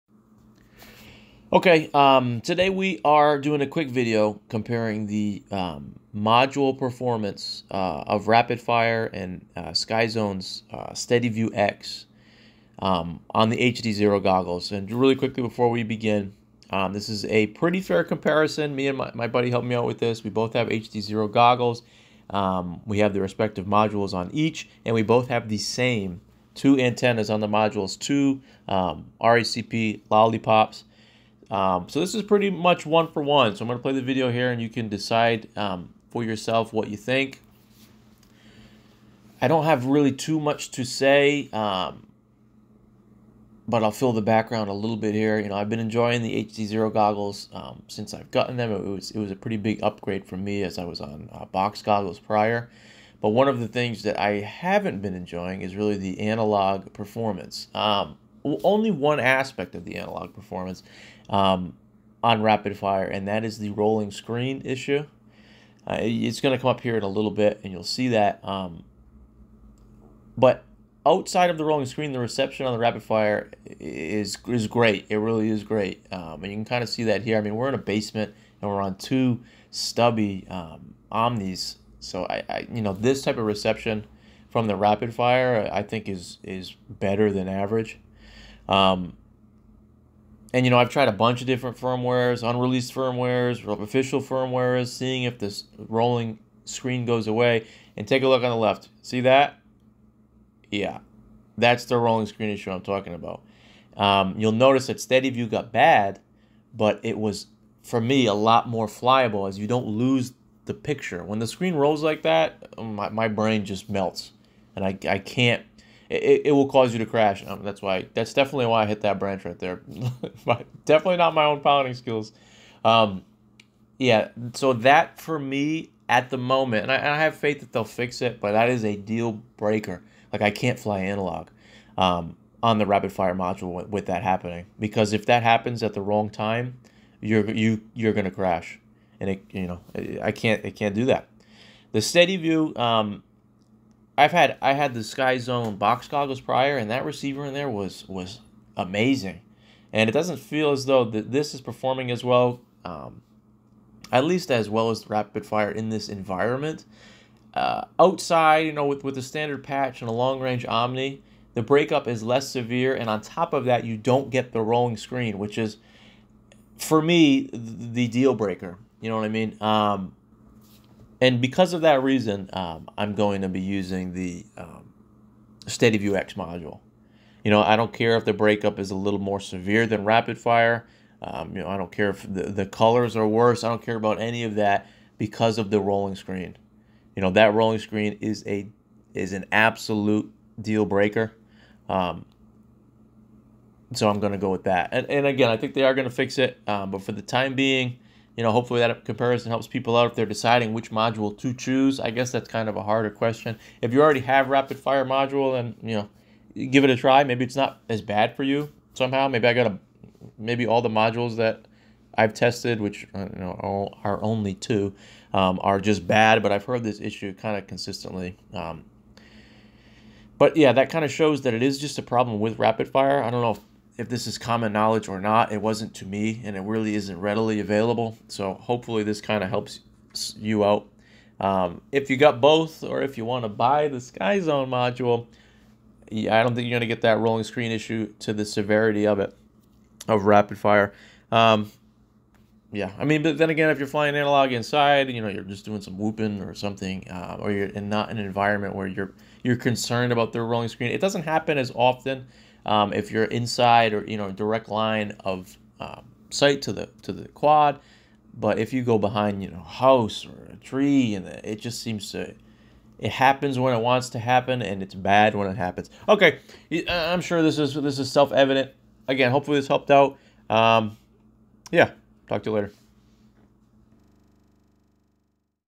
Analog Module Comparison for the HDZero Goggles - (Rapidfire vs Steadview X)
Hopefully this helps some pilots decide which they rather chose for their HDZero Goggles. All firmware's (Goggles and Modules) are the latest at time recording (4/29/23)

Thanks to @biscuitfpv for helping me with this